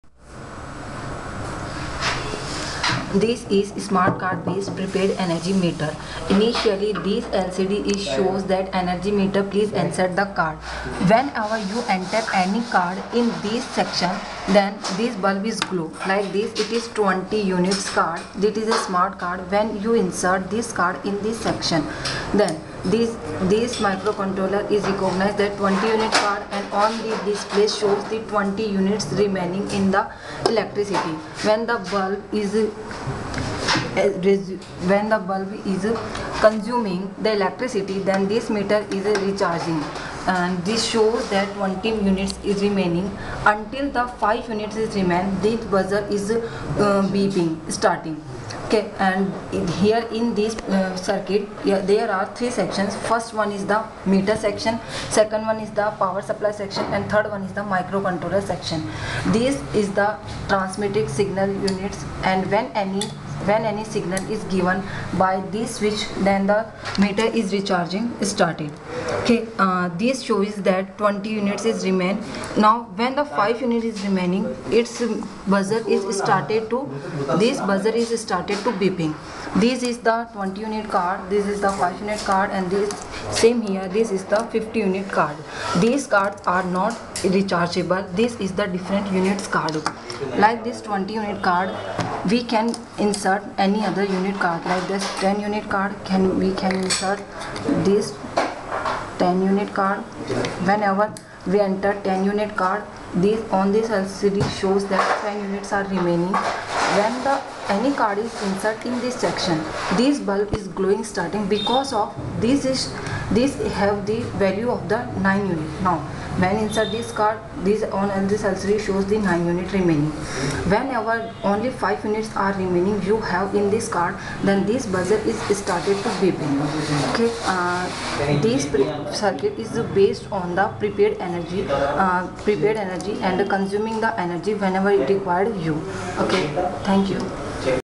Electricity Billing System Using Smart Card
Prepaid Energy Meter has been proposed as an innovative solution aimed at facilitating affordability and reducing the cost of utilities. This mechanism, essentially, requires the users to pay for the electricity before its consumption. In this way, consumers hold credit and then use the electricity until the credit is exhausted. If the available credit is exhausted then the supply of electricity is cutoff by a relay.At first an energy meter is to be made. The output of the energy meter works as the input to the next circuit for prepaid services.